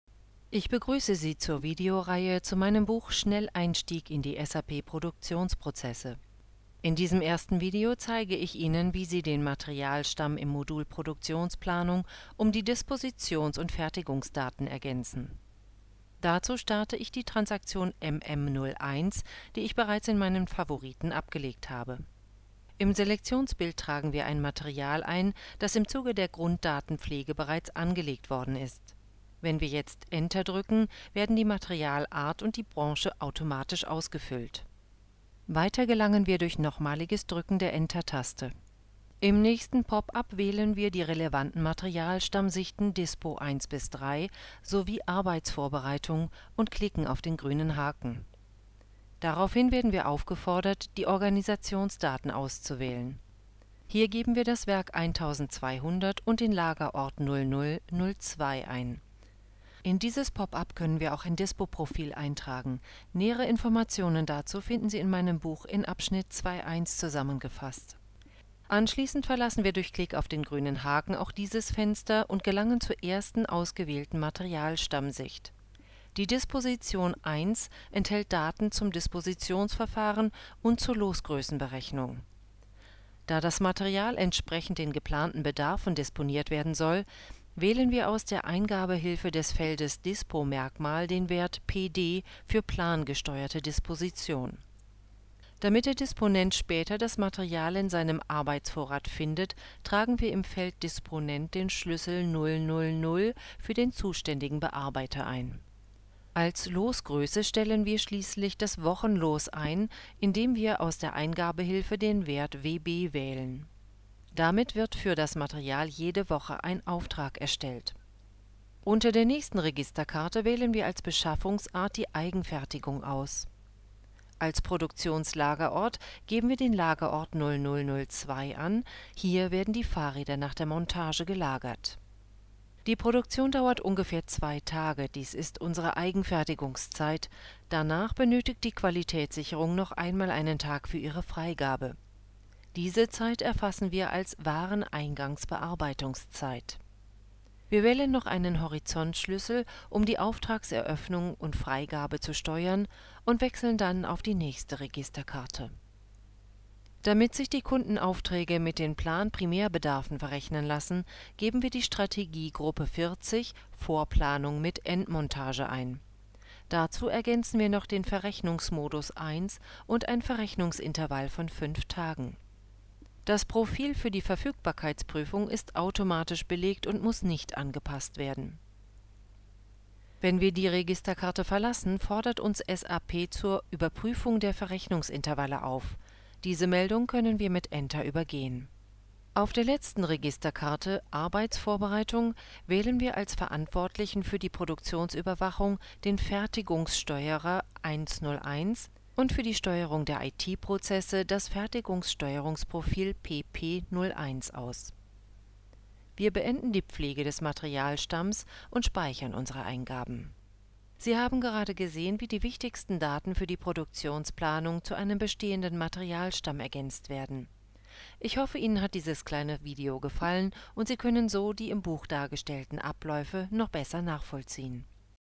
Schnelleinstieg in die SAP-Produktionsprozesse (PP)
Guten Tag , in diesem Video zeige ich Ihnen, wie Sie den Materialstamm im SAP ERP Modul PP (Produktionsplanung) um die Dispositions- und Fertigungsdaten ergänzen.
Dazu starte ich die SAP Transaktion MM01, die ich bereits in meinen Favoriten abgelegt habe. Im Selektionsbild trage ich ein Material ein, das im Zuge der Grunddatenpflege
bereits angelegt worden ist. Wenn ich jetzt Enter drücke, werden die Materialart und die Branche automatisch ausgefüllt. Wir starten die Transaktion.
Im nächsten Pop-up wählen wir  die relevanten SAP ERP Materialstammsichten „Dispo 1 bis 3“ sowie „Arbeitsvorbereitung“ und gehen weiter .
Jetzt werden wir aufgefordert, die Organisationsdaten auszuwählen. Hier geben wir das Werk „1200“ und den Lagerort „0002“ ein.
In dieses Pop-up können wir auch ein Dispoprofil eintragen. Nähere Informationen dazu finden Sie in meinem Buch „Schnelleinstieg in die SAP-Produktionsprozesse".
Anschließend verlassen wir durch Klick auf den grünen Haken auch dieses Fenster und gelangen zur ersten ausgewählten Materialstammsicht. Die „Disposition 1“ enthält Daten zum Dispositionsverfahren und zur Losgrößenberechnung.
Da das Material entsprechend den geplanten Bedarfen disponiert werden soll, wählen wir aus der Eingabehilfe des Feldes „Dispomerkmal“ den Wert „PD“ für plangesteuerte Disposition.
Damit der Disponent später das Material in seinem Arbeitsvorrat findet, tragen wir im Feld „Disponent“ den Schlüssel „000“ für den zuständigen Bearbeiter ein.
Als Losgröße stellen wir schließlich das Wochenlos ein, indem wir aus der Eingabehilfe den Wert „WB“ wählen. Damit wird für das Material jede Woche ein Auftrag erstellt.
Unter der nächsten Registerkarte wählen wir als Beschaffungsart die „Eigenfertigung“ aus. Als Produktionslagerort geben wir den Lagerort „0002“ an – hier werden die Fahrräder nach der Montage gelagert .
Die Produktion dauert ungefähr 2 Tage – dies ist unsere Eigenfertigungszeit -  und danach benötigt die Qualitätssicherung noch einmal  einen Tag für ihre Freigabe.  Diese Zeit erfassen wir als Wareneingangsbearbeitungszeit.
Wir wählen noch einen Horizontschlüssel, um die Auftragseröffnung und -freigabe zu steuern und wechseln dann auf die nächste Registerkarte.
Damit sich die Kundenaufträge mit den Planprimärbedarfen verrechnen lassen, geben wir die Strategiegruppe „40 – Vorplanung mit Endmontage“ ein. Dazu ergänzen wir noch den Verrechnungsmodus „1“ und ein Verrechnungsintervall von „5 Tagen “.
Das Profil für die Verfügbarkeitsprüfung ist automatisch belegt und muss nicht angepasst werden.
Auf der letzten Registerkarte „Arbeitsvorbereitung“ wählen wir den Fertigungssteuerer „101“  und das Fertigungssteuerungsprofil „PP01“ aus.
Wir beenden die Pflege des Materialstamms und speichern unsere Eingaben. Sie haben gerade gesehen, wie die wichtigsten Daten für die Produktionsplanung zu einem bestehenden Materialstamm ergänzt werden. Ich hoffe, Ihnen hat dieses kleine Video gefallen, und Sie können so die im Buch dargestellten Abläufe noch besser nachvollziehen.